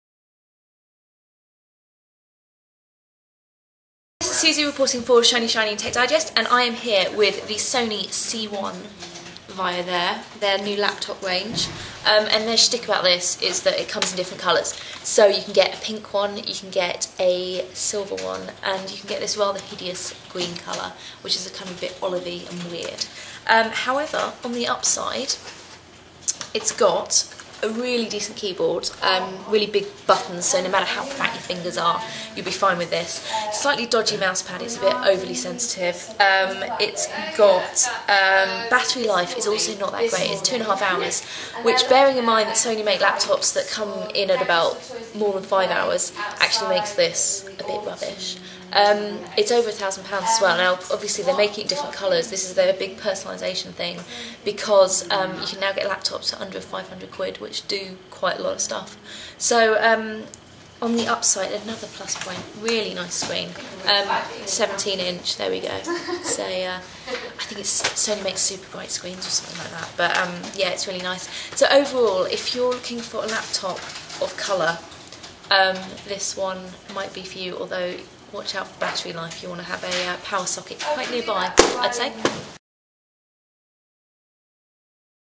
One Minute Video Review: Sony C1 Vaio
A look at the new C1 Vaio range from Sony, featuring the Olive green one